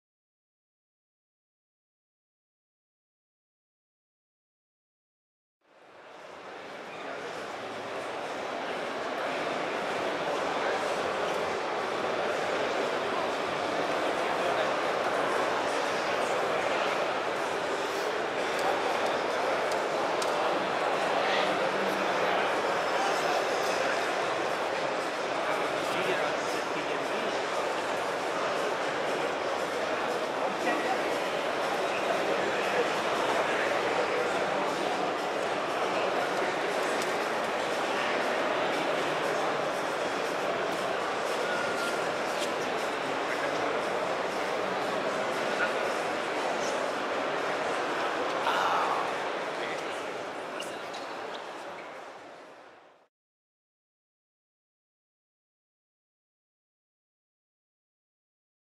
BMW 528i Hartge (E28) - Retro Classics 2023 - Stuttgart
A BMW 528i Hartge (E28) equipped with a M106 turbo engine of a 745i (E23) at the Retro Classics 2023 in Stuttgart (Germany).

-  HP
-  Nm max. torque
-  km/h topspeed